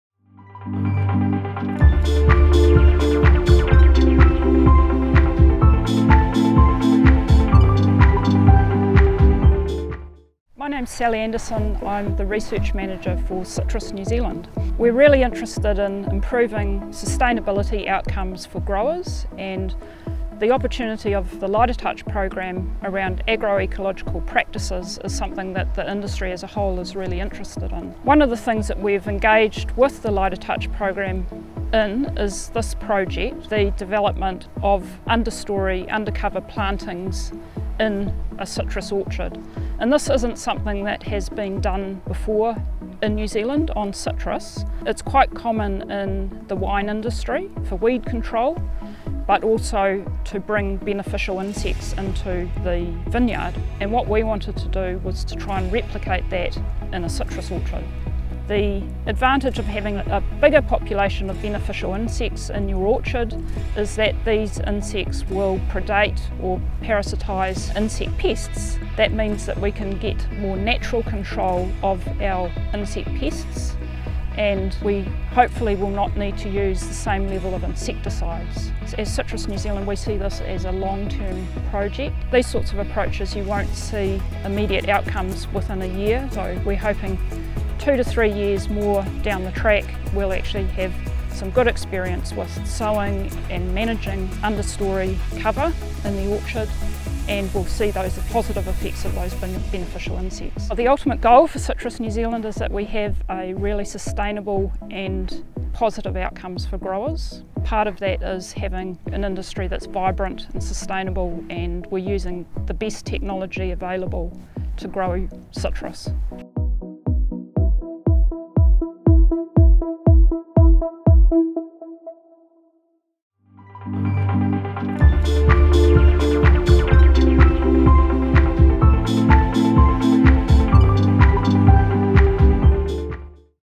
ALT Gisborne Citrus Project - Citrus NZ's involvement
Citrus NZ research manager Sally Anderson explains Citrus NZ's interest in this A Lighter Touch programme is about helping to improve sustainability outcomes for citrus growers.